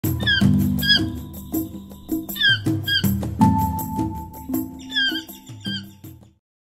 Amazon Toucan - Wildlife.
Welcome to another video from the Nature Relaxing channel.

This is the Amazon Toucan, also known as Toco Toucan, it is the largest of the toucans and its main feature is its huge and colorful beak, which can reach up to 22 centimeters in length. They are found in the forests of South America and are considered one of the most striking bird symbols in Brazil.

Thanks, good vibes!

Nature Relaxing